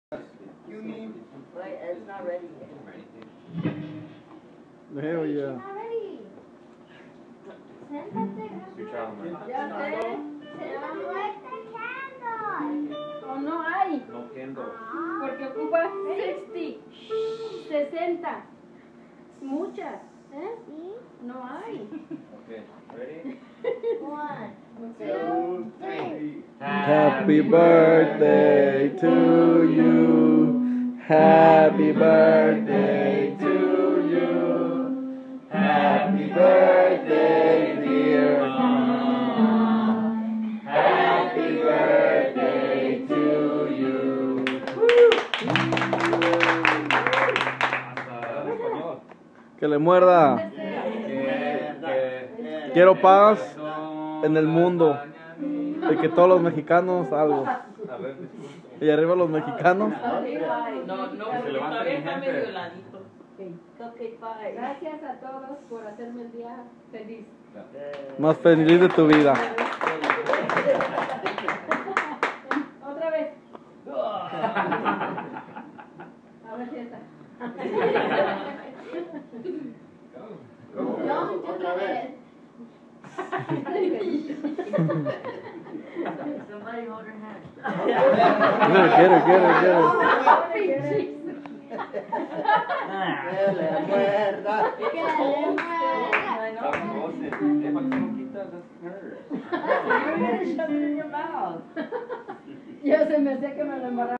Feliz Cumpleaños a mi jefa
The family enjoying my moms bday